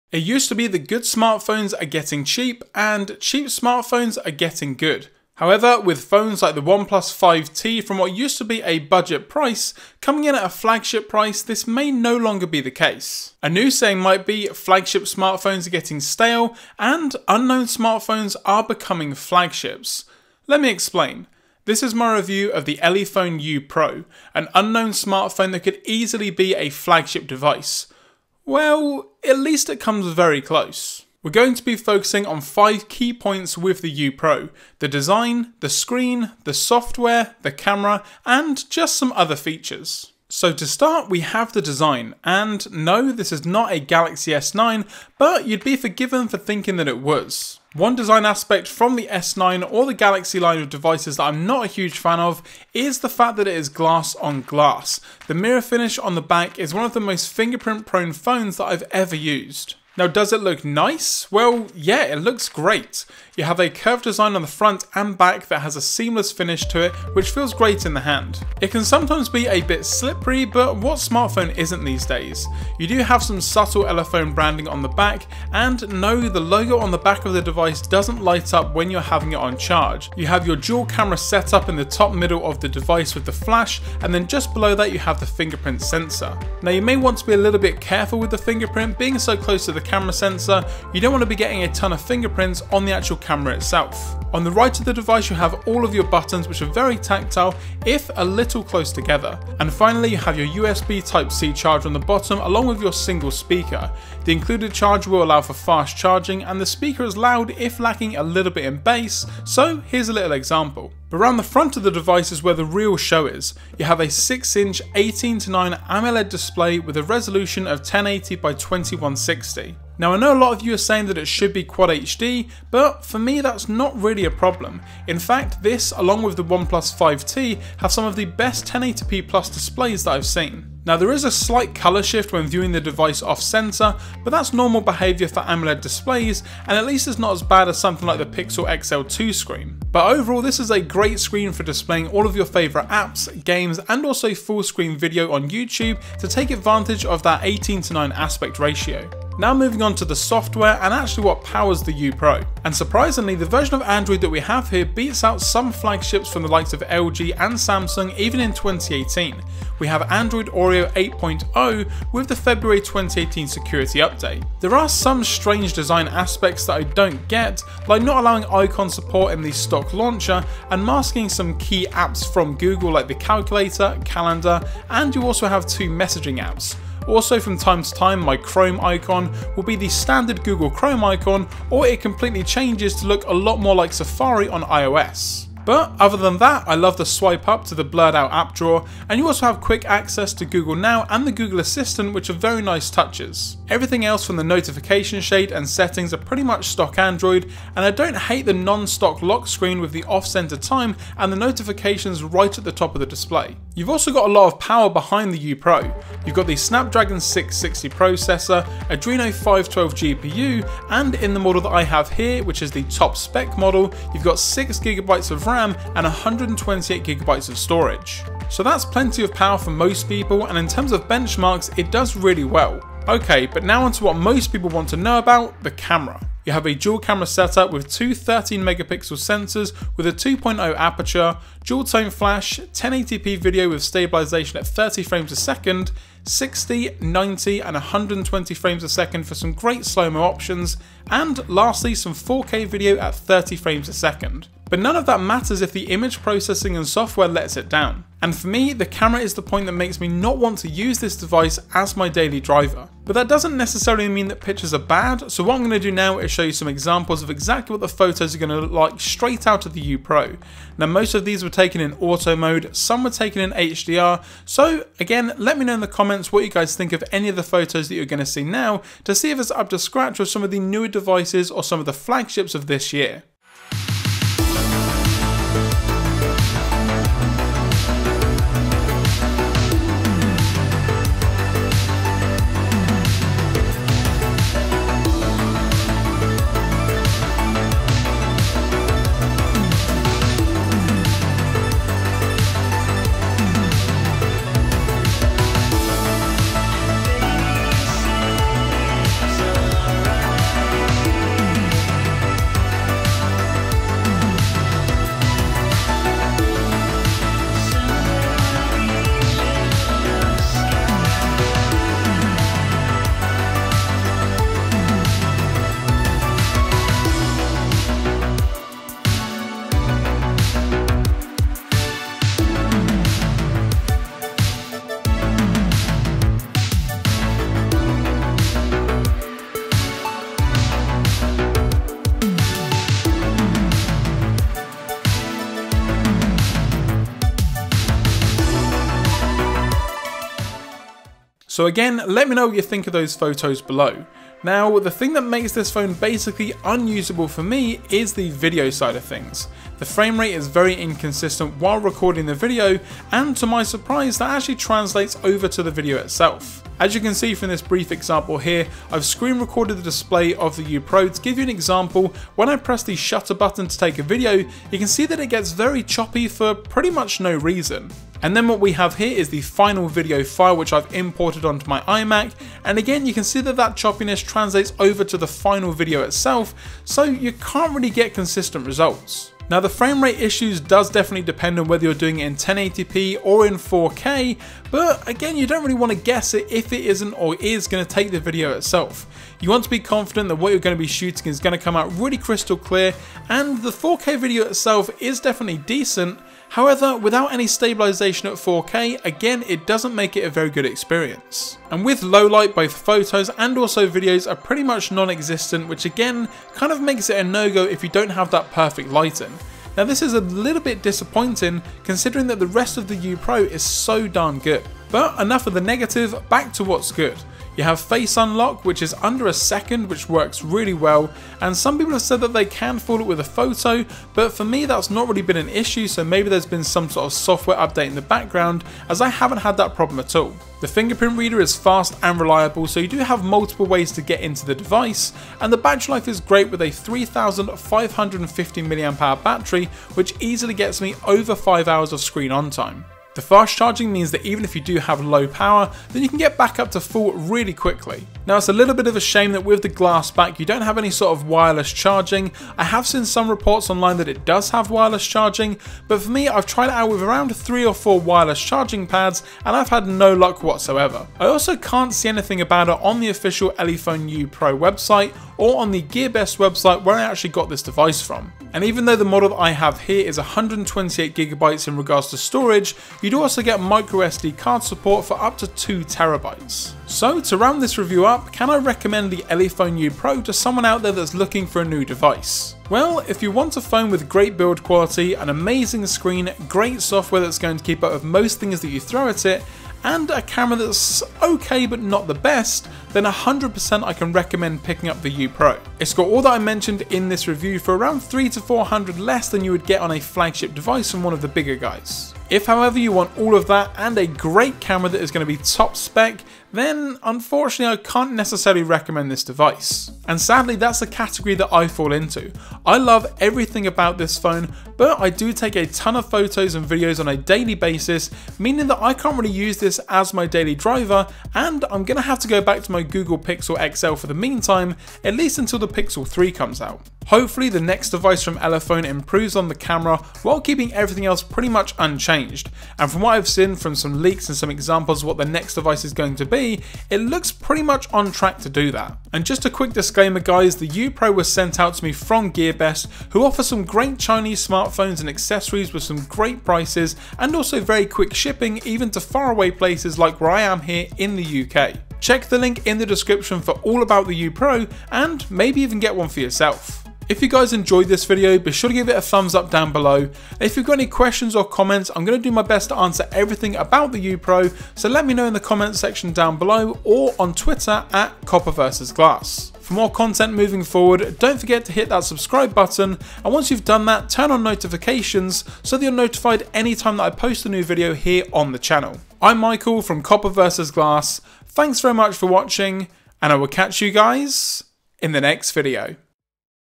Elephone U Pro In Depth Review!
In this video we review the Elephone U Pro. This is a great phone for the price and the performance is great. If your looking for an awesome device with only really one set back then this is for you. Lets take a look.

LINKS